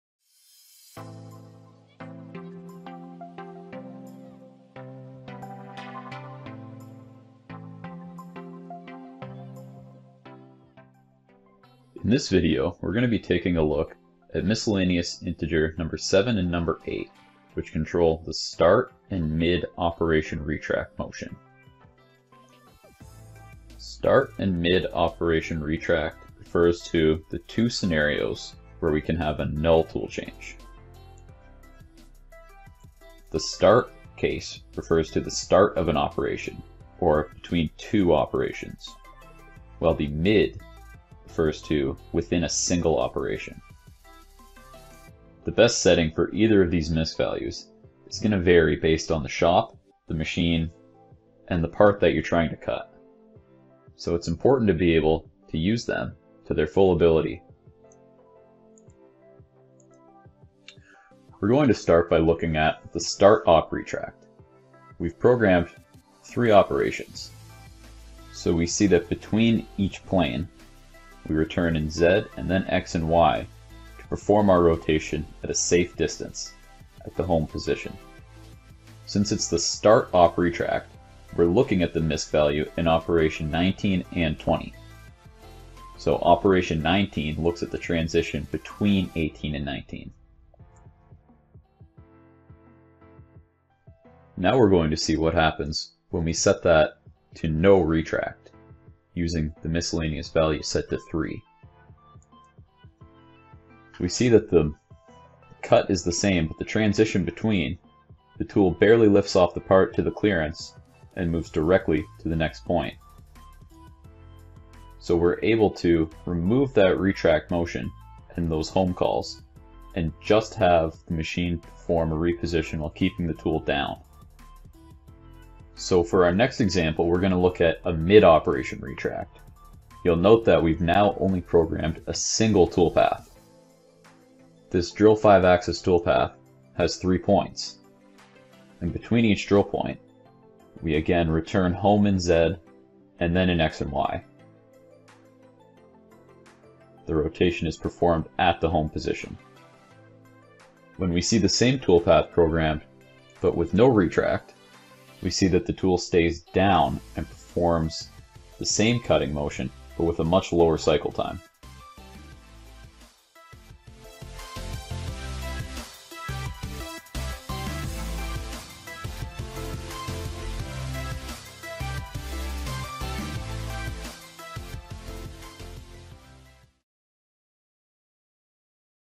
Controlling Multi-Axis Retract Motions With IKE Post
Watch as our post developer Alex walks you through some Misc Value options with your IKE Post to help further control your retract moves in multi-axis toolpathing within Mastercam.